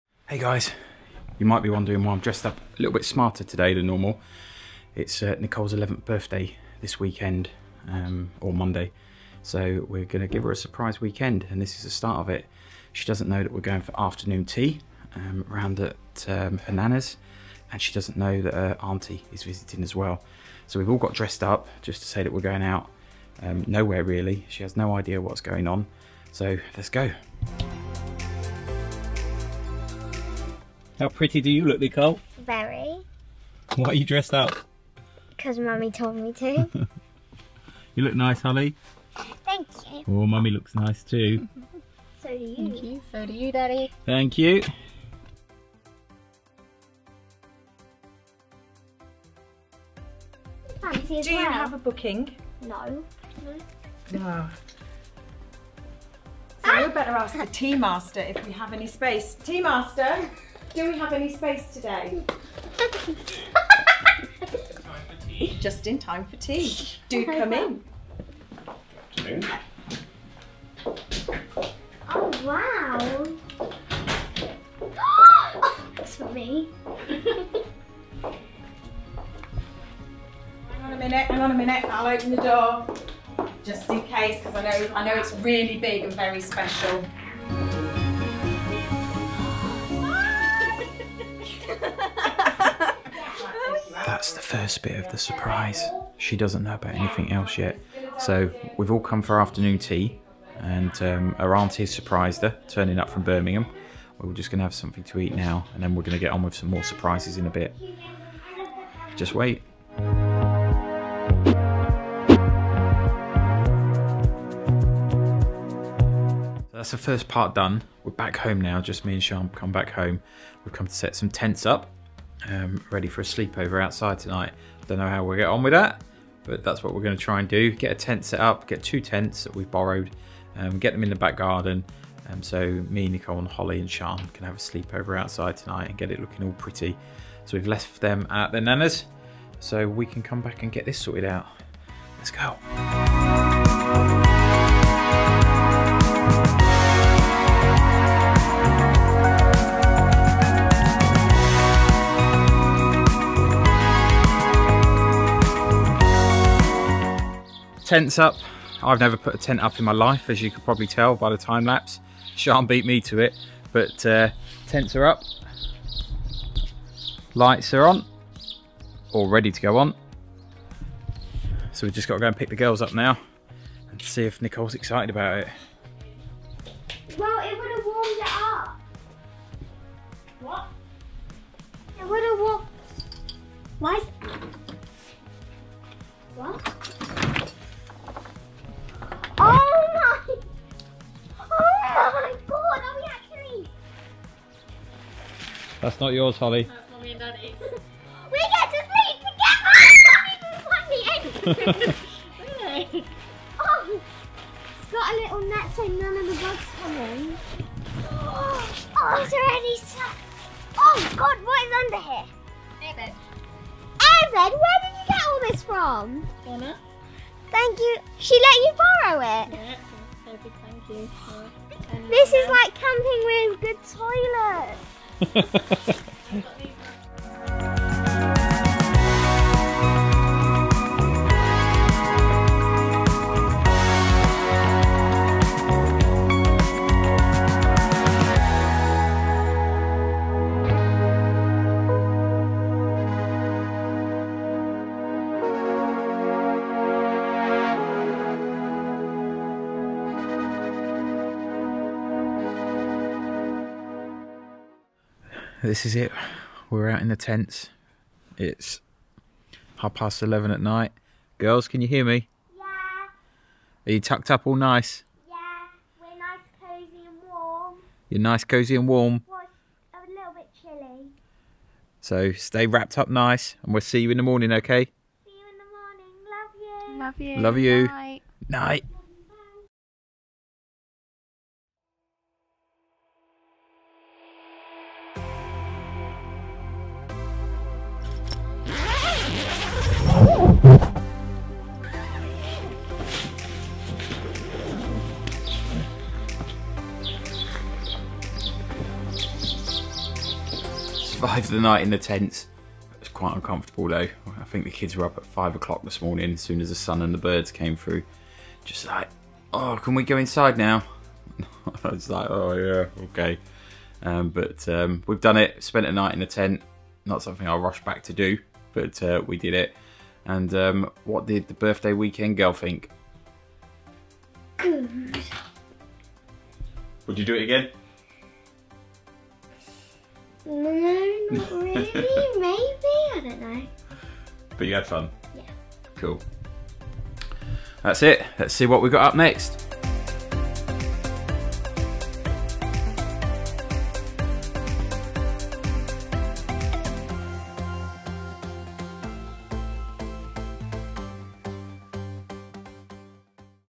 Camping Surprise Part 1
Its a special Birthday weekend with three days of surprises. Trying to make it a fun and special birthday with not being able to have a party and see all of her friends. So follow our three day birthday special.